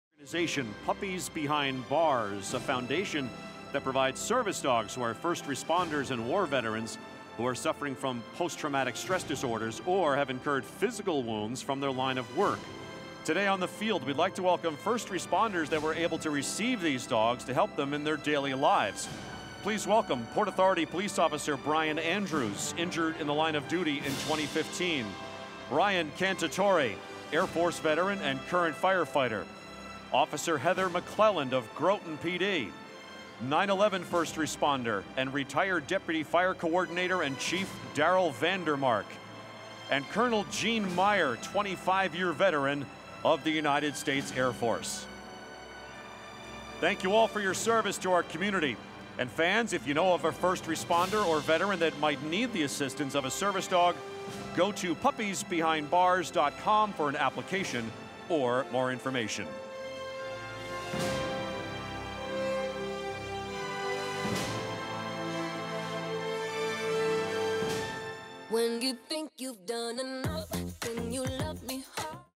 NY GIANTS & Puppies Behind Bars Honoring First Responders & Military Veterans12-29-2019
NY GIANTS & Puppies Behind Bars Honoring First Responders & Military Veterans Service Dogs and their Partners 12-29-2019. Puppies Behind Bars and the New York Giants hosted their First Responders & Military veterans at the GIANTS VS. EAGLES Game on 12/29/2019

Darryl Vandermark & Patriot